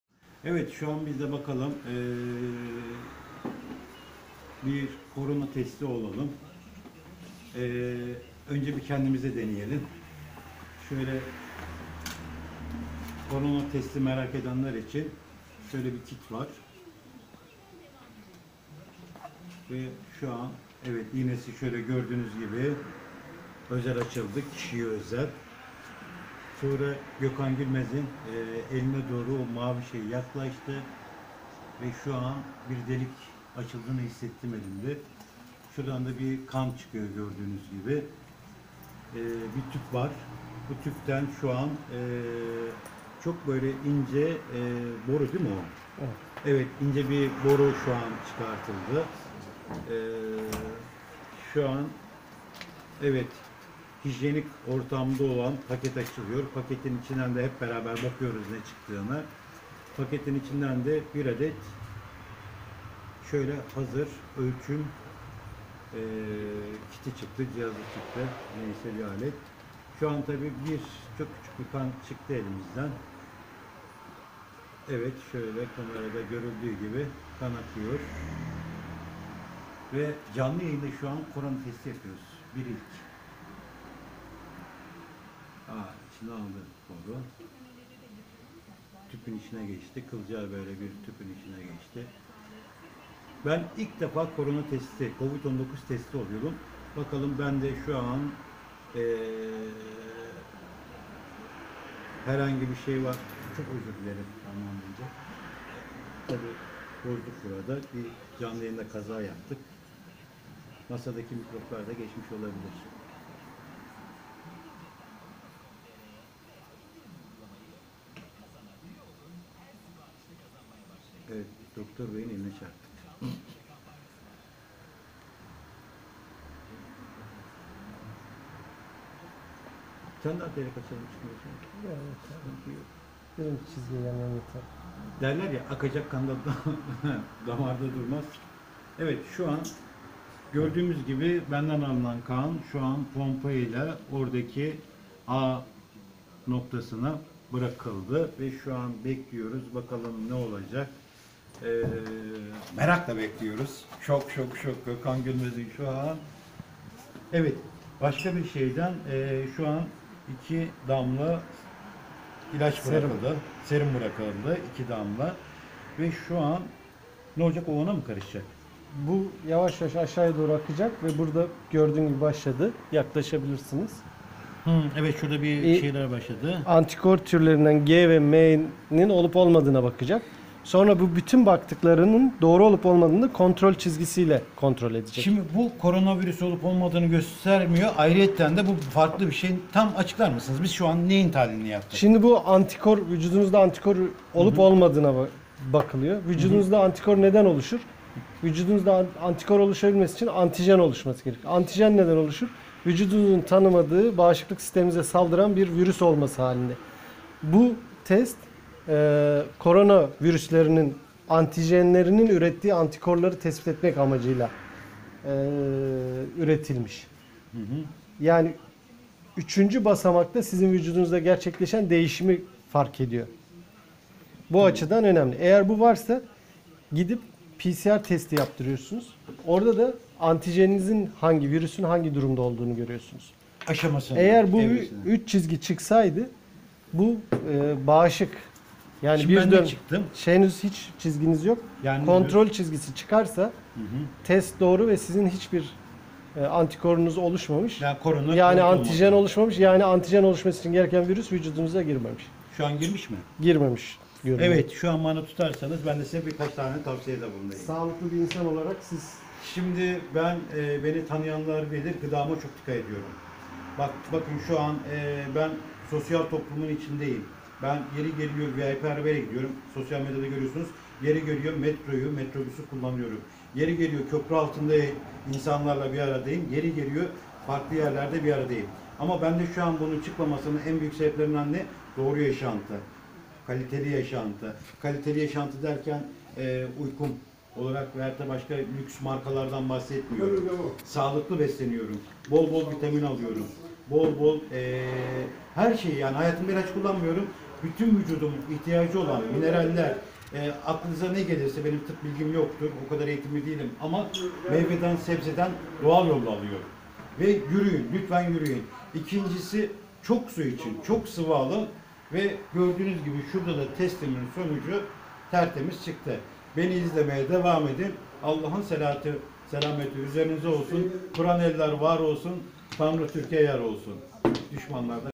Gökhan Gülmez Korona Kovit 19 Testi yaptırdım
Gökhan Gülmez Korona Testi yaptırdım. Negatif çıktı.Sosyal mesafe kurallarına dikkat edelim.DSÖ ve Sağlık Bakanlığımızın tavsiyelerine uyalım. Maskemizi takalım.Evde kal Türkiyem.